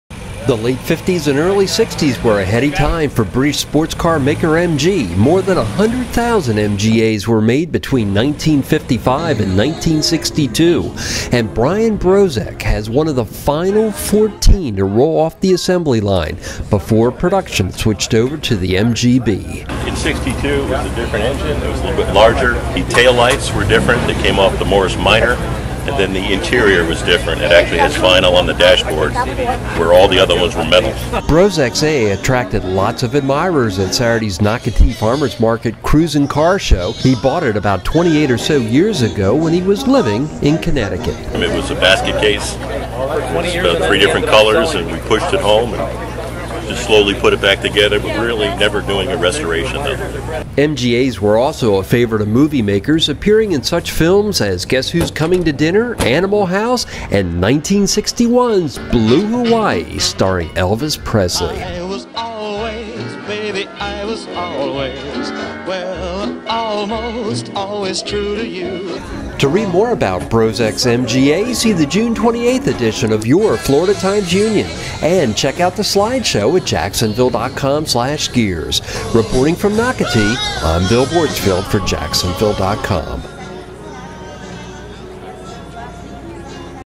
First Coast Gears: 1961 MGA
The late 50s and early 60s were a heady time for British sports car maker MG. Meet Brian Brozeck and his '62 MGA and see what they have in common with Elvis.